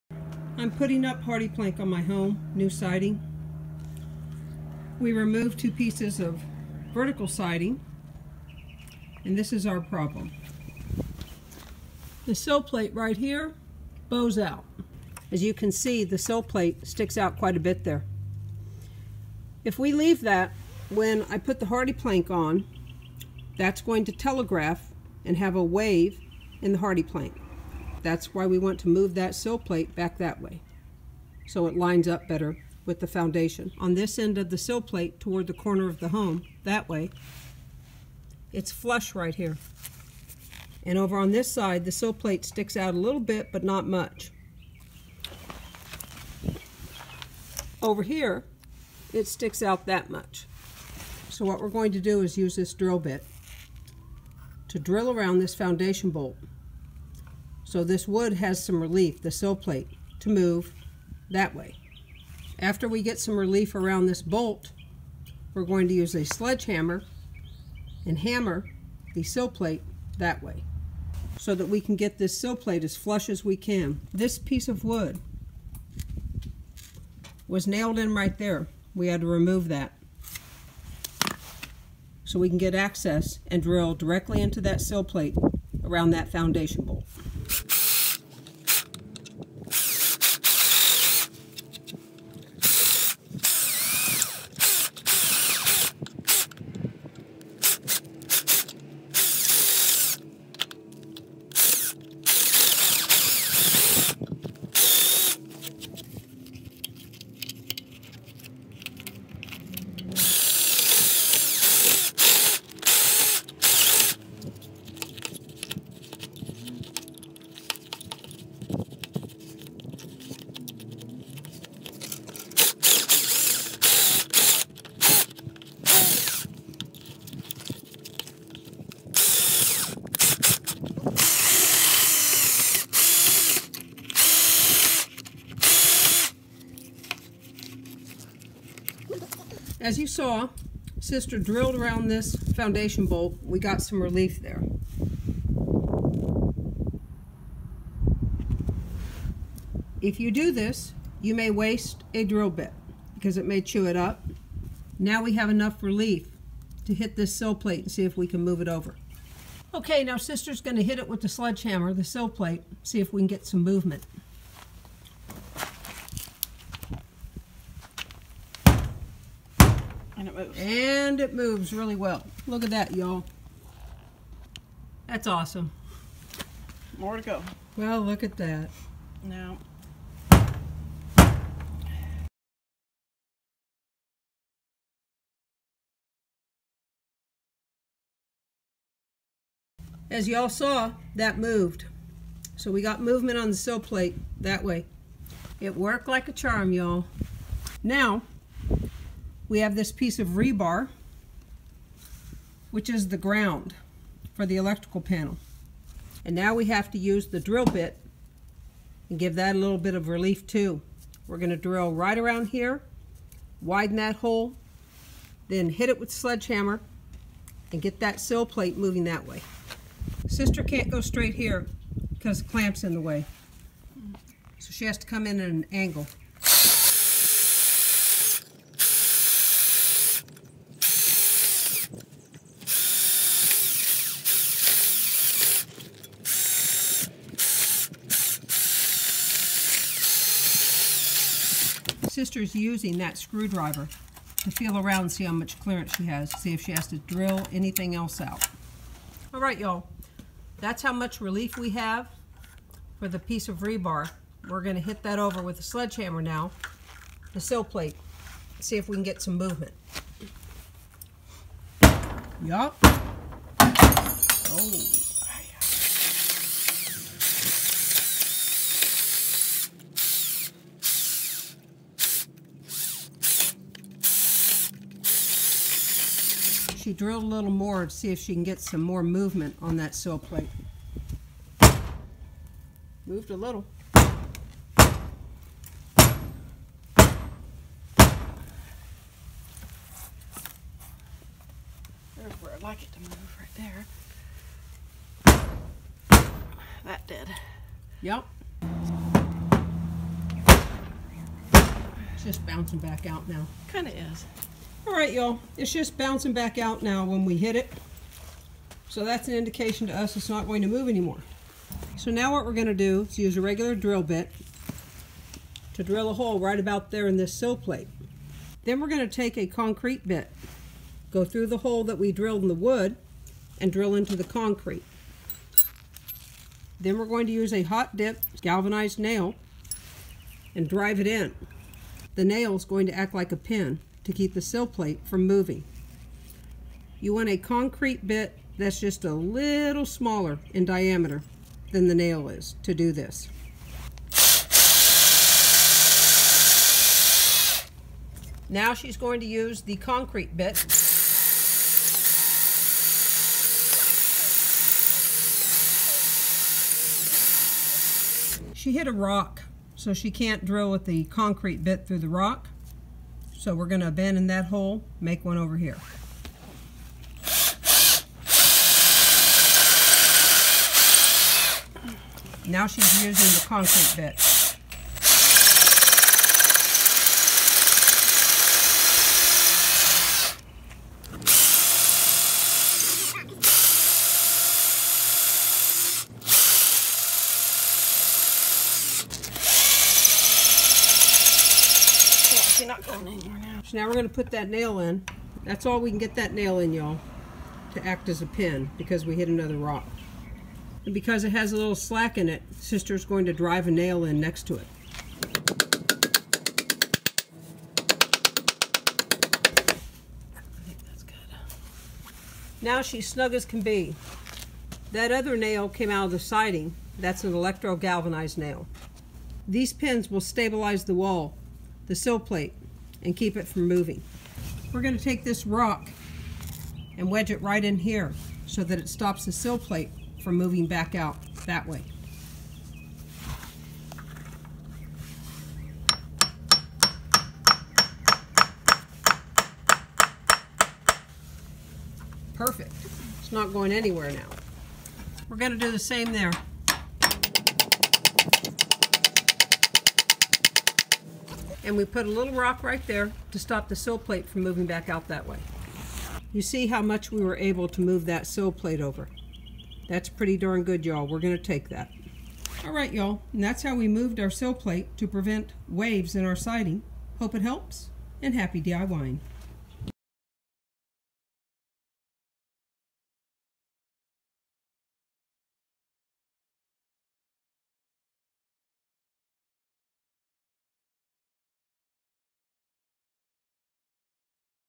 Move Sill Plate That Overhangs Foundation Wall
Sill plate not flush with the foundation wall or overhangs the foundation wall?  We drilled around a foundation bolt and then used a sledge hammer to move the sill plate flush with the foundation wall, so the Hardie Plank lap siding will not have waves in it.  If the sill plate overhangs the foundation wall, lap siding and vertical siding will have waves in it.